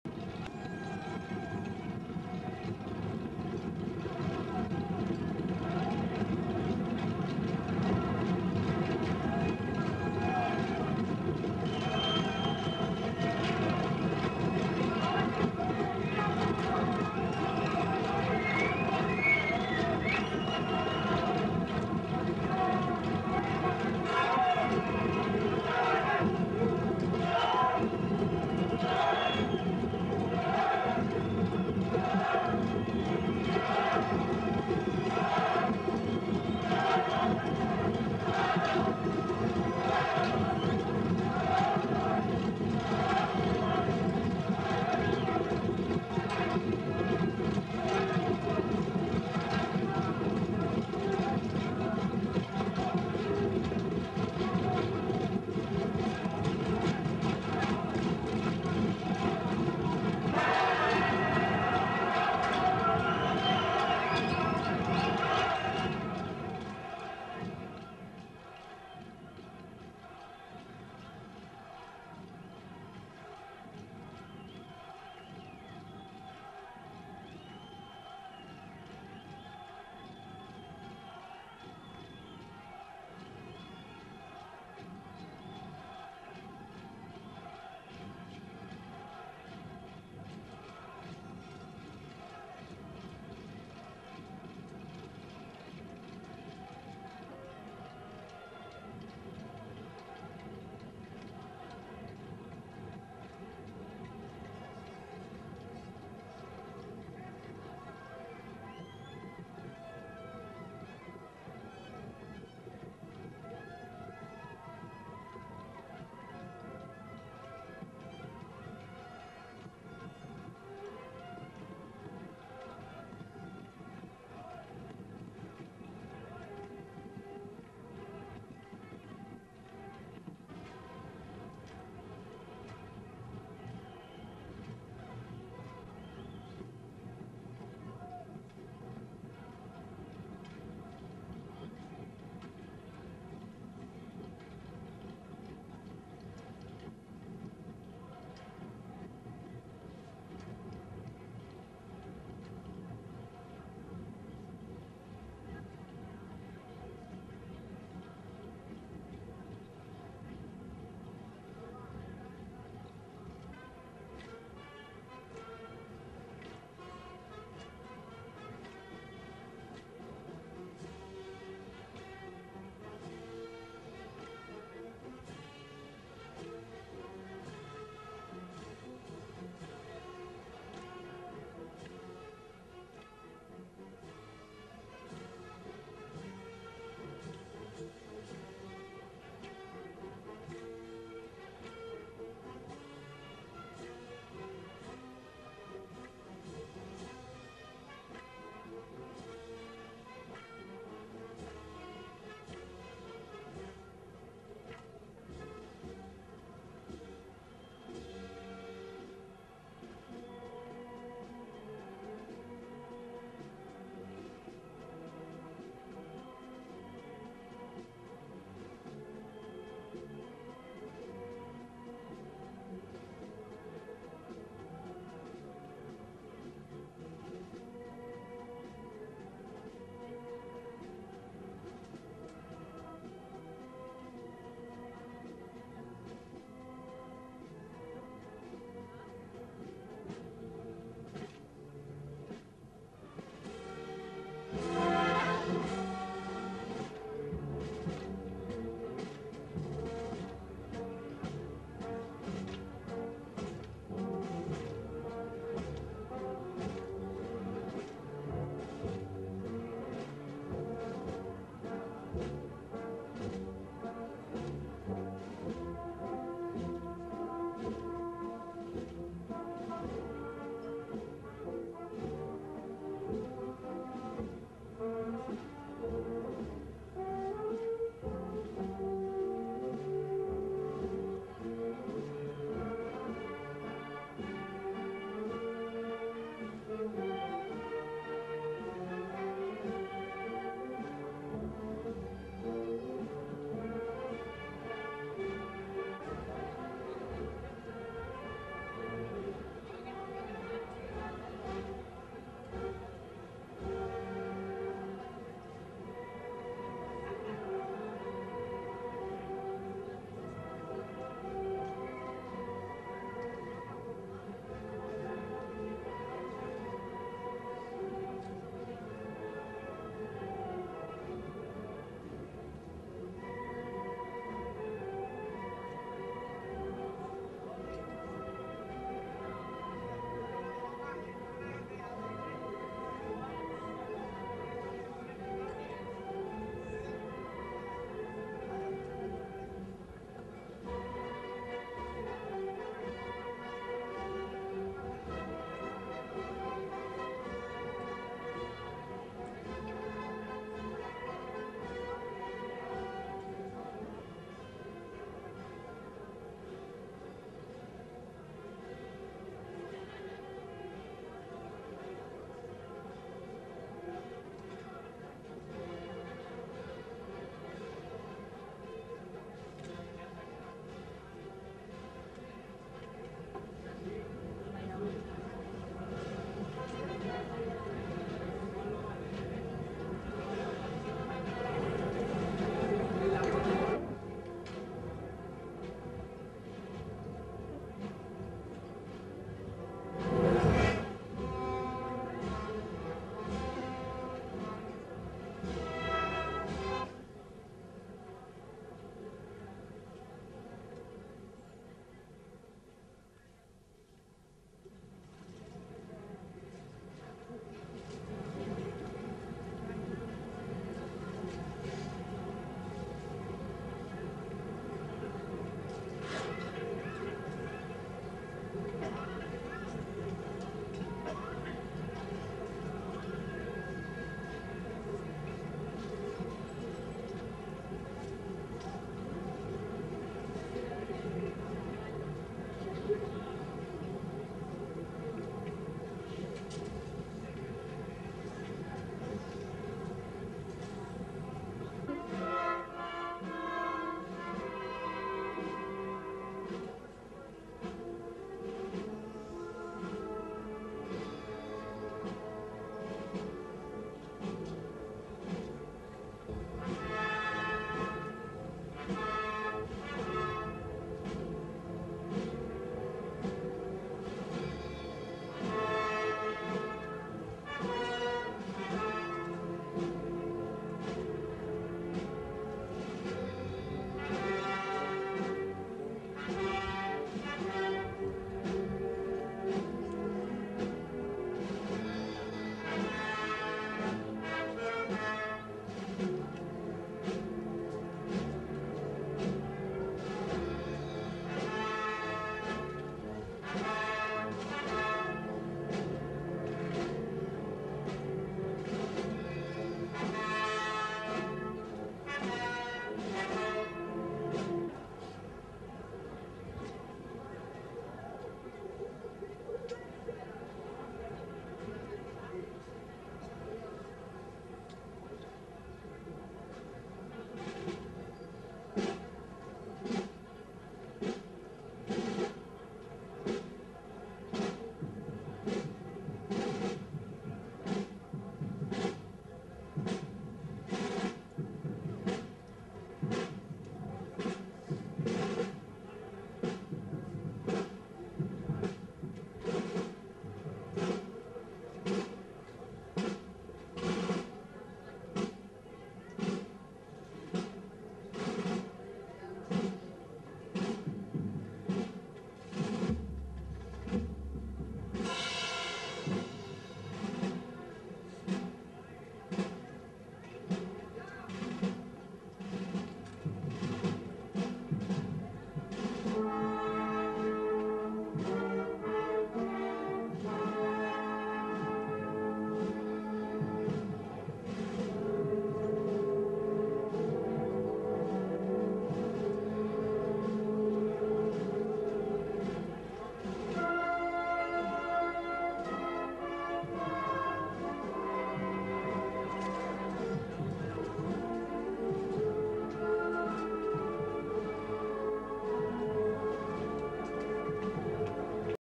Camino del Calvario. 15. 1989-2000. Semana Santa de Cuenca.
Semana Santa de Cuenca. Años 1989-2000. Viernes Santo. Procesión Camino del Calvario. Autor: Gregorio Cubillo.